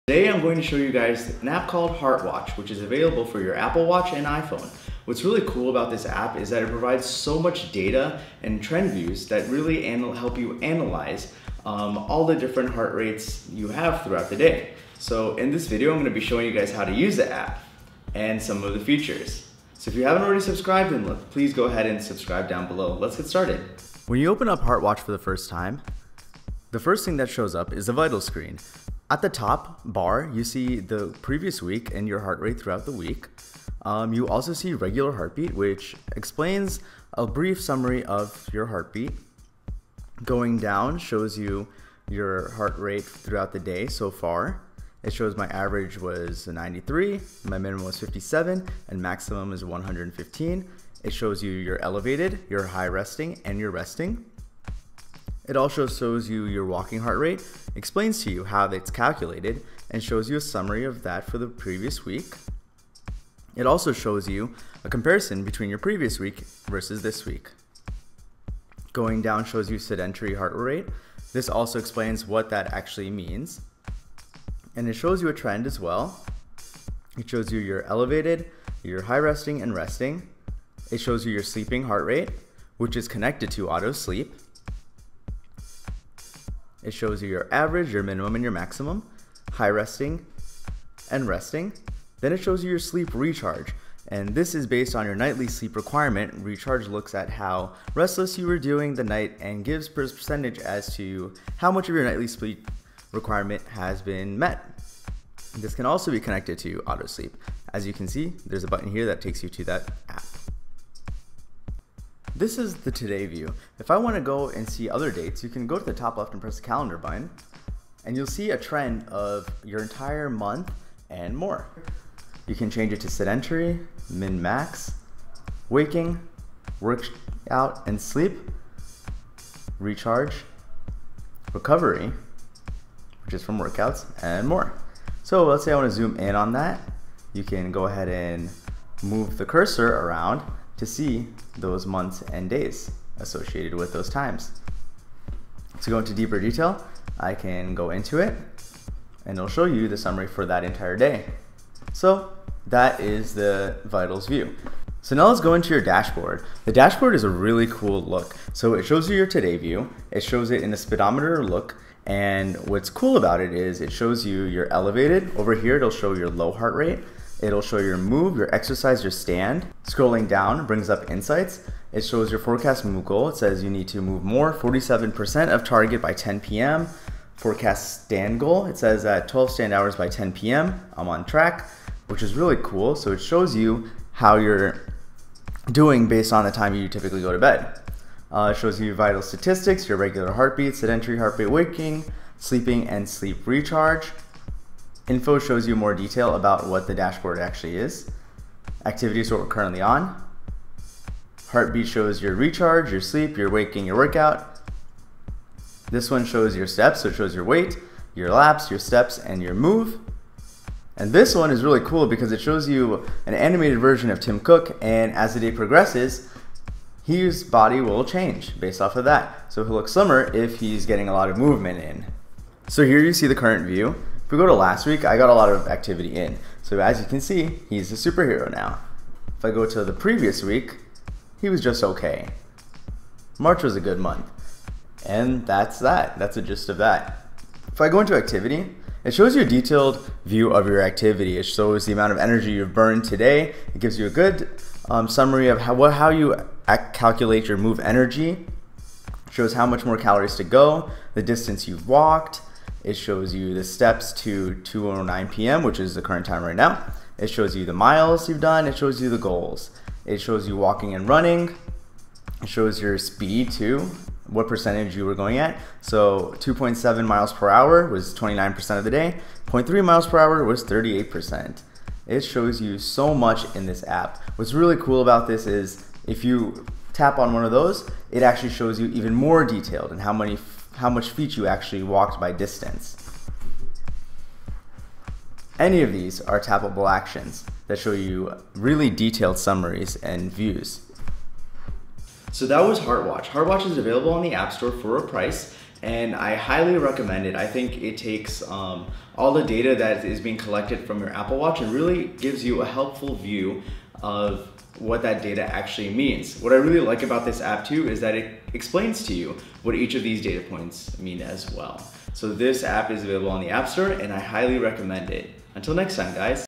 HeartWatch | Heart Analysis for Apple Watch!
In this video, I show you all the HeartWatch app for Apple Watch! This app gives an in depth analysis of your heart rate based on readings from your Apple Watch!